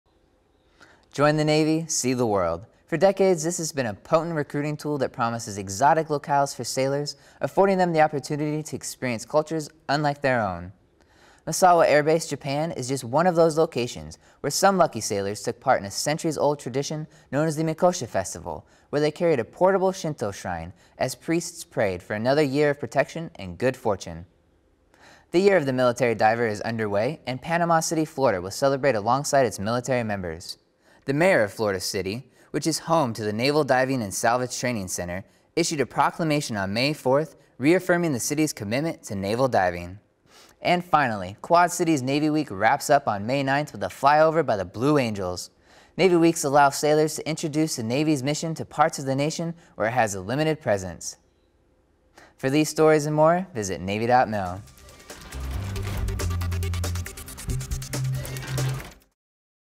Headlines for Friday May 8, 2015 (HL 8)
Sailors take part in Japanese tradition. Celebrating the Year of the Military Diver. Navy Week Quad Cities wraps up.